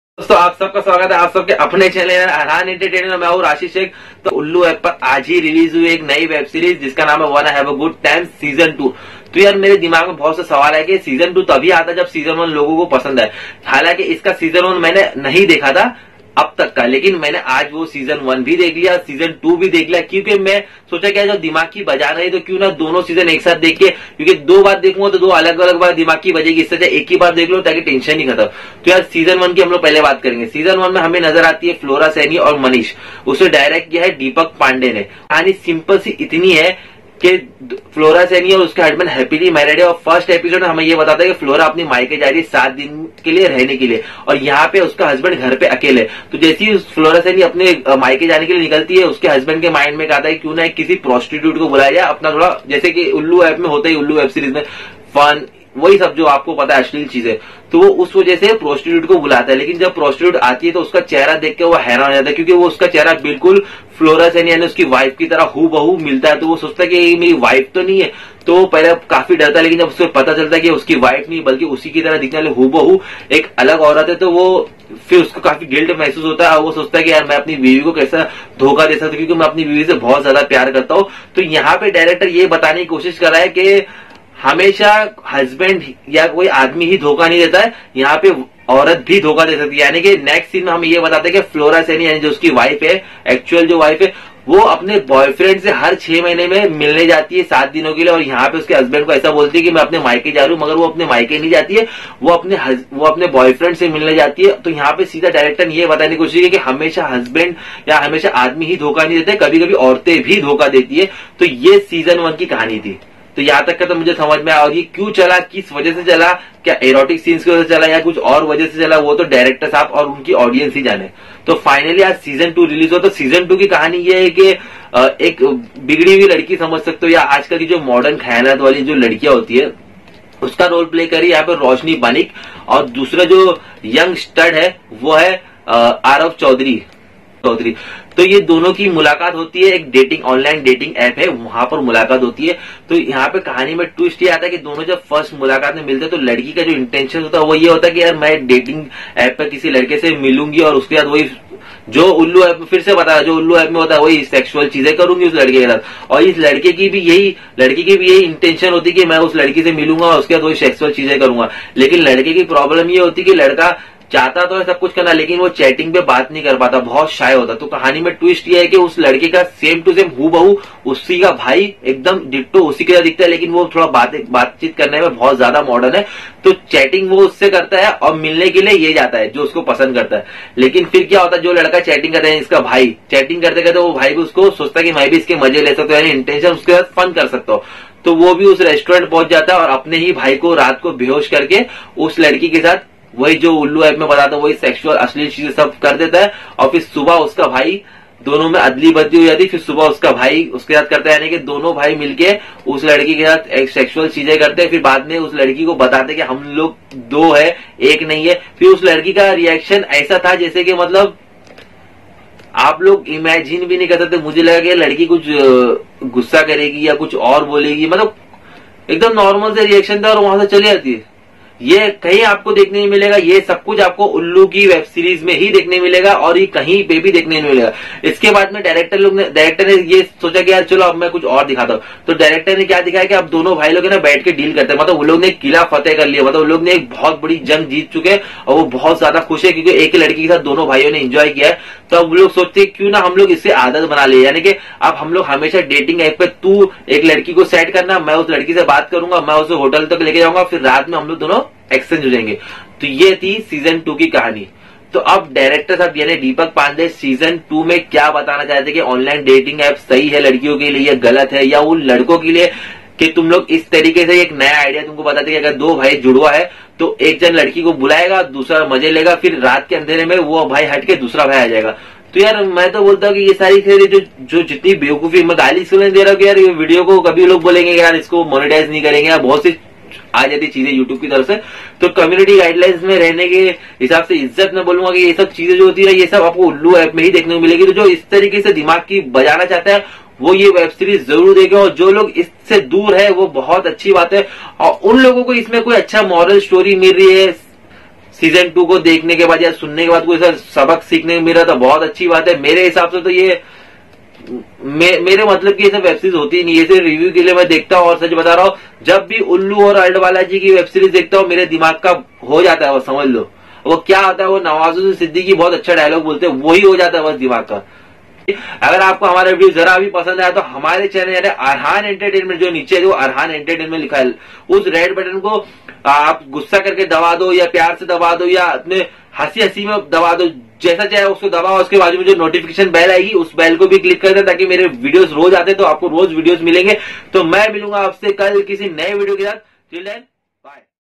Wanna Have A good time Season 1 and 2 Review By Arhaan entertainment
Hi dosto aaj hum Review karegey Wanna Have A good time Season 1 and 2 Review by Arhaan entertainment

   ARHAAN ENTERTAINMENT
By profession I m-Embroidery Designer(Art).